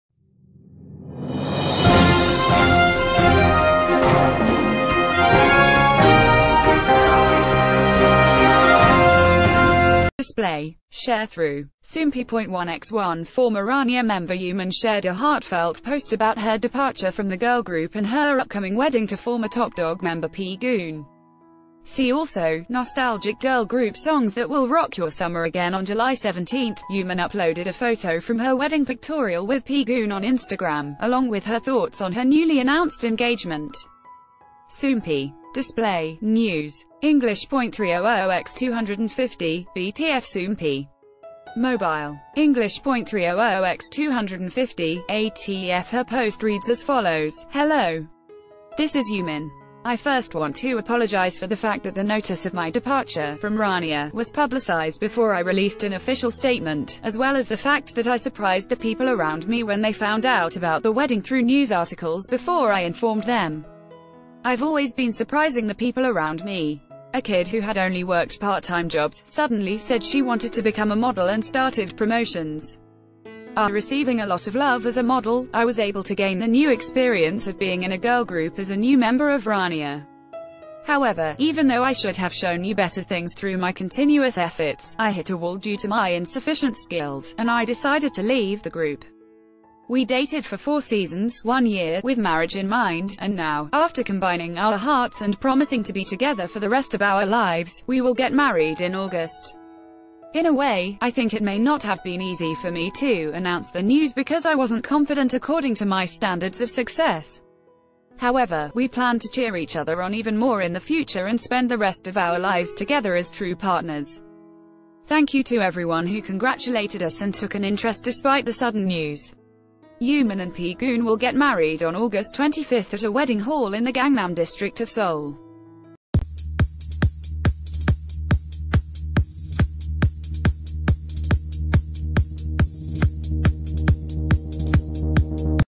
Former Rania Member Yumin Shares Candid Post Ahead Of Marriage To Former ToppDogg Member P-Goon
Display. Sharethrough. Soompi.1x1 Former Rania member Yumin shared a heartfelt post about her depart...